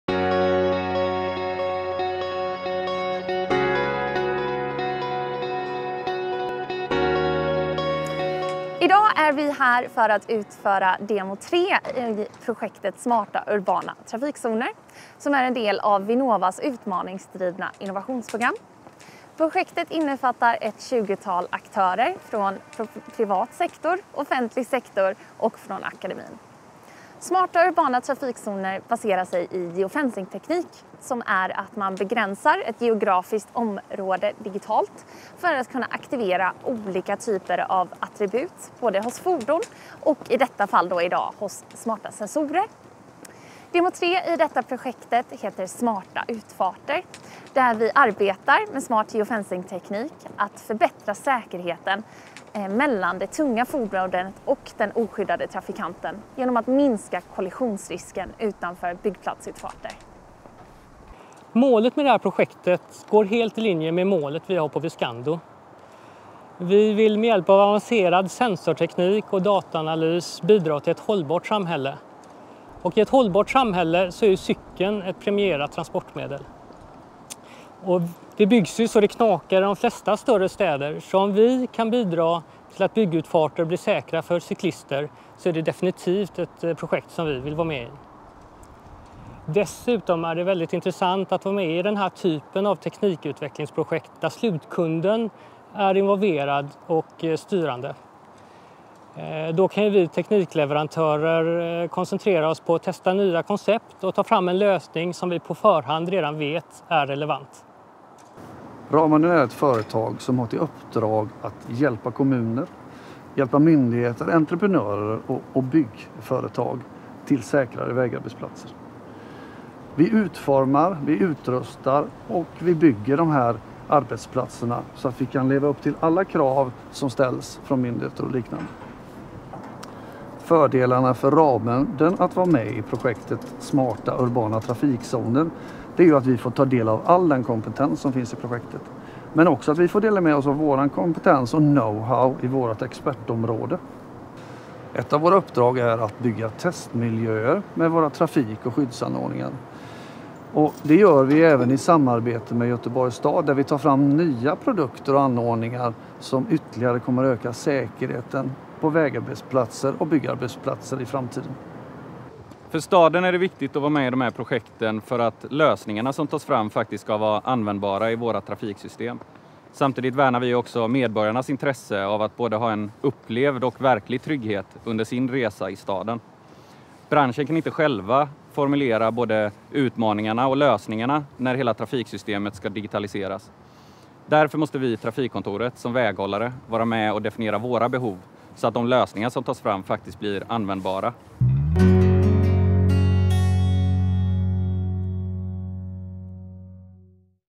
Smarta Urbana Trafikzoner - Smarta Utfarter
Smarta urbana trafikzoner kommer att vara en del av den framtida flexibla staden där fordon rör sig på människors villkor. Målet är att Smarta urbana trafikzoner blir ett kraftfullt verktyg, baserat på geofencing, för att bidra till tystare, säkrare och hälsosammare miljöer att leva och bo i.

Denna del av projektet syftar till att demonstrera utformningsförslag och tekniska lösningar för smarta zoner vid byggutfarter, med huvudfokus är säkerhetshöjande åtgärder för framförallt cyklister.